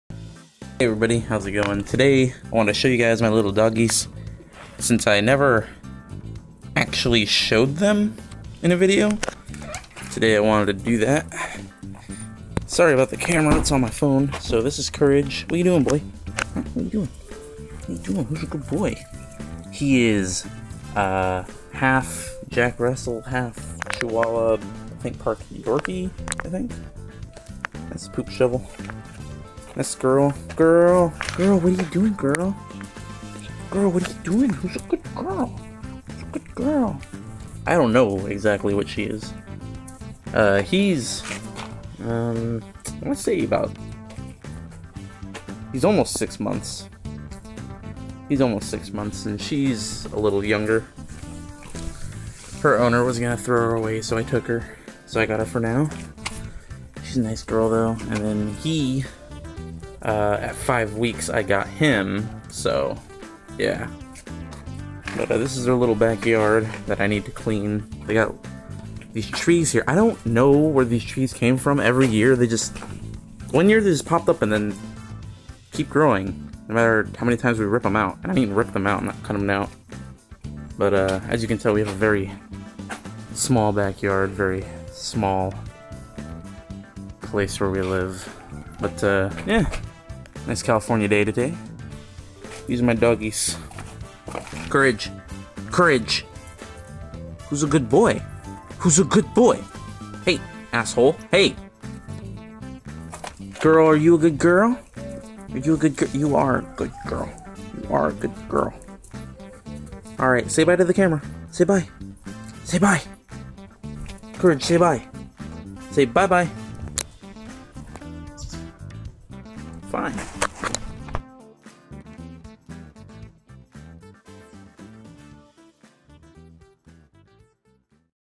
Courage and Girl
These are my dogs Courage, and Girl, they're adorable.